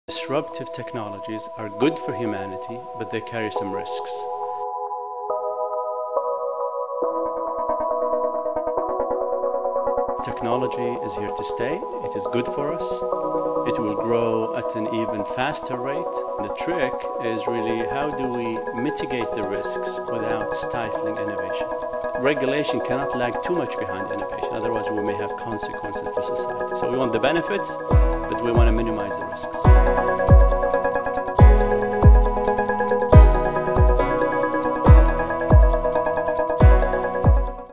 Nayef Al-Rodhan: How should we approach technology?
We caught up with Professor Al-Rodhan right before his KAPTalks in Nicosia at University of Cyprus.